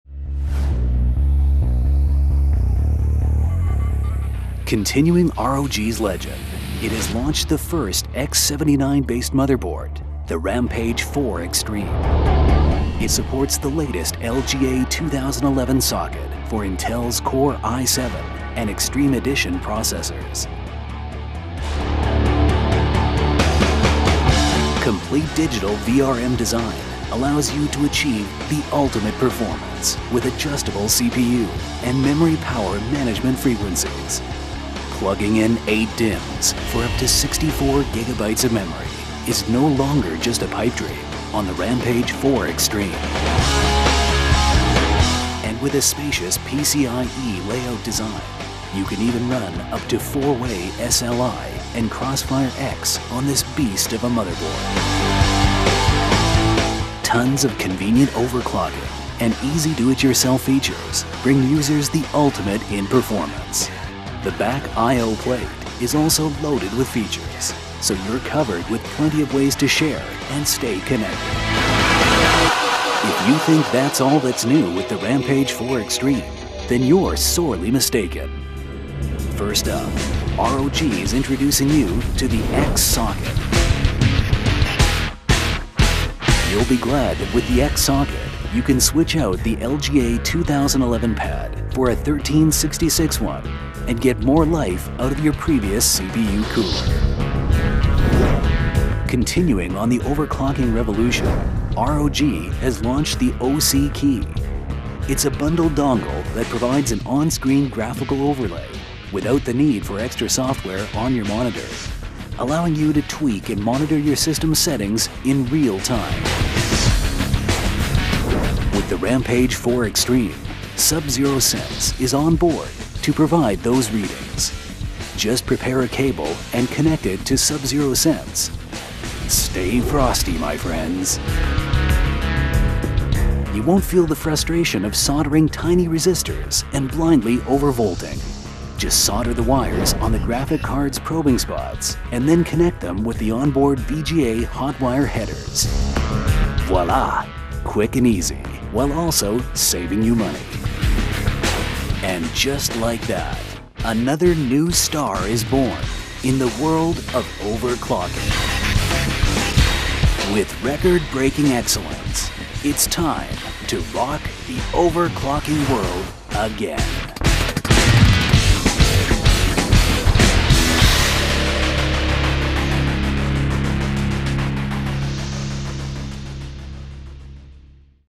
ASUS Rampage IV Extreme (X79 LGA2011) product video - official demo (1080p)
Az ASUS számos X79 (LGA2011) alaplapot jelentett be a Sandy Bridge-E processzorokhoz, viszont még a P9X79 Deluxe is csak szürke eminenciás a gyártó ROG szériás Rampage IV Extreme és Formula megoldásához képest. Összefoglalónkban az ASUS játékosoknak szánt X79 lapjaival foglalkozunk.